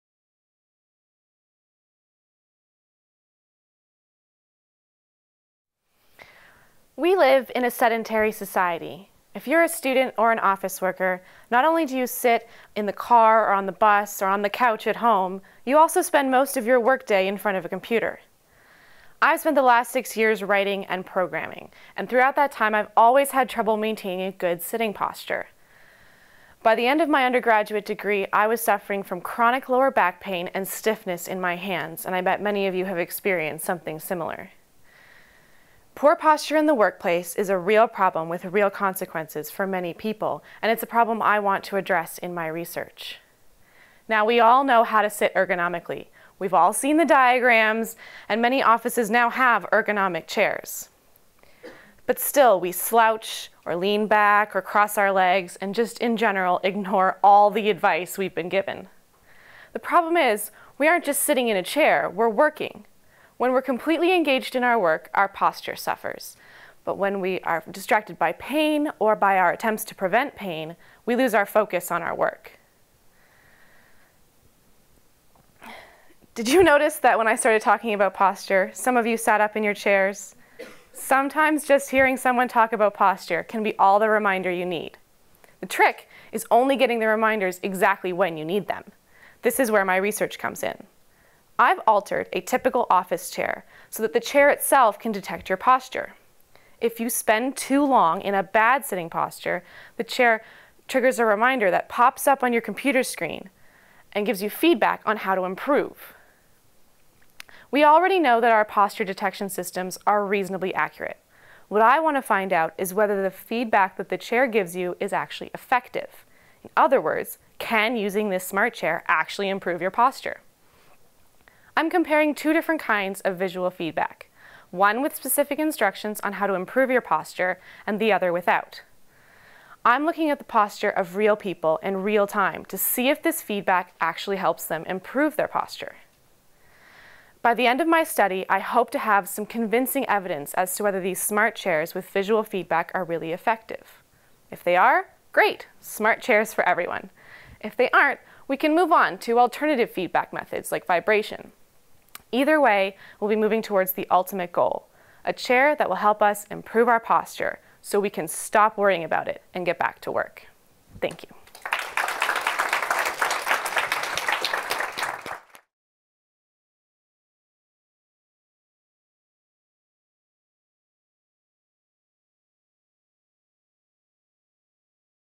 2015 3MT Carleton U. Finalist - Jessica Speir
Jessica Speir was a finalist in the 2015 Carleton Three Minute Thesis competition (held on March 3). A student in the Human-Computer Interaction master's program, Jess spoke on "Can feedback from a smart chair really improve your posture?"

The 3MT competition challenges graduate students to present the main idea of their master's or PhD research in three minutes or less. Judges evaluate participants on their ability to effectively and succinctly communicate their research to a general audience. For more information on 3MT at Carleton,